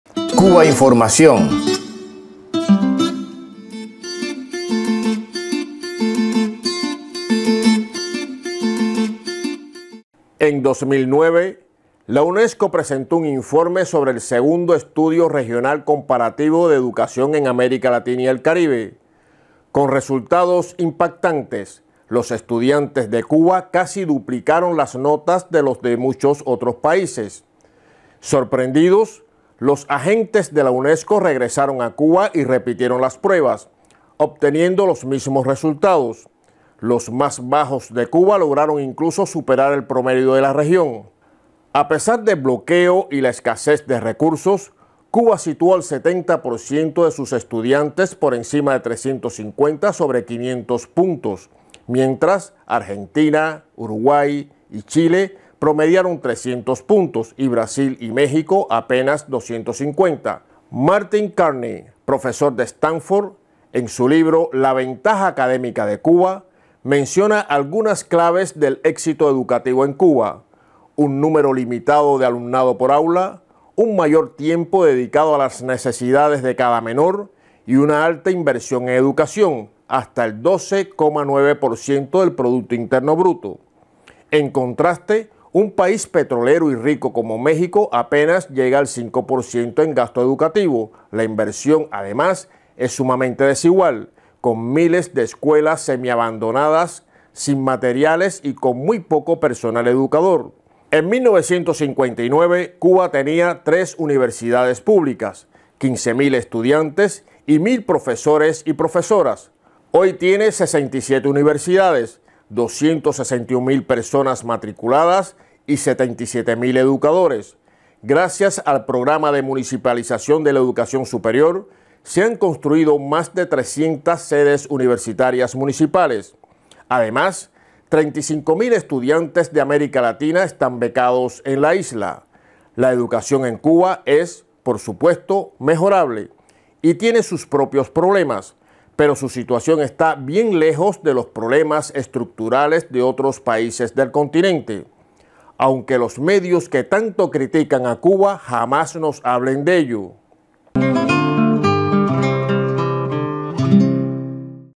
La superioridad educativa de Cuba en América Latina está respaldada por la UNESCO
Cubainformación TV -- Basado en un texto de Tatiana Coll -- La Jornada (México) / Blog "Isla mía" (Cuba).- En 2009, la Unesco presentó un informe sobre el "Segundo estudio regional comparativo de educación en América Latina y el Caribe", con resultados impactantes: los estudiantes de Cuba casi duplicaron las notas de los de muchos otros países.